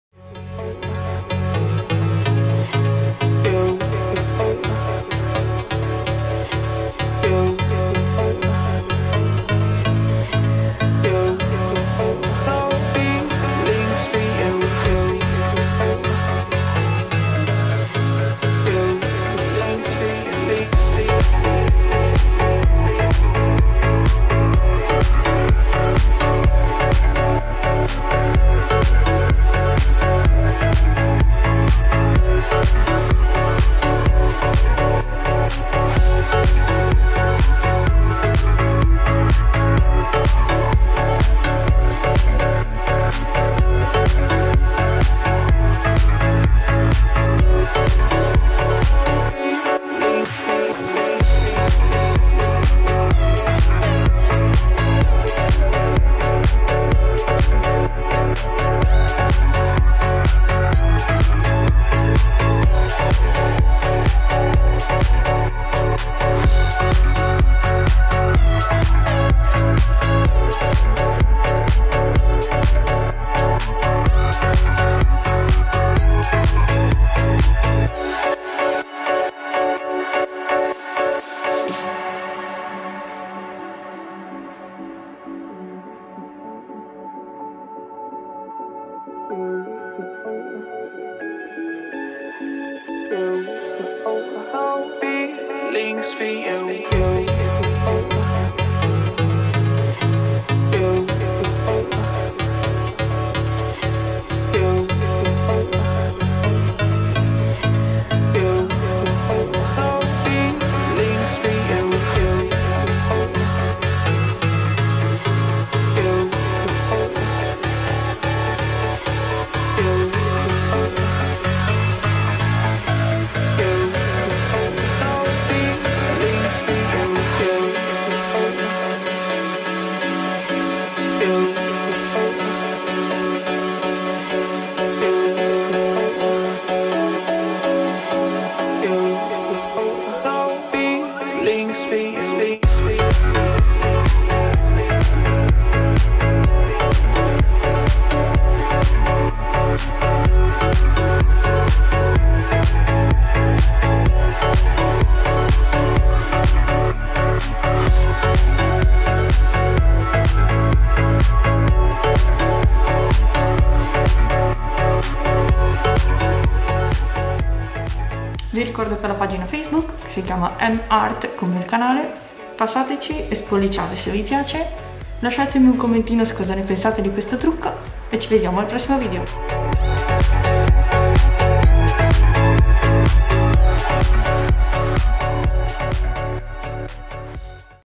Halloween Makeup: Monster Zombie | Trucco per Halloween - M'Art - HD
Halloween! La serata horror per eccellenza!
Non so neanche io come definire questo makeup.. però mi piace tantissimo! Spero possa piacere anche a voi!

Lasciatemi scritto nei commenti come vi travestirete ad Halloween!

A presto un altro video!!

Votate il video positivamente se vi è piaciuto! Condividetelo con chiunque!

Grazie di ❤ ;)
M'Art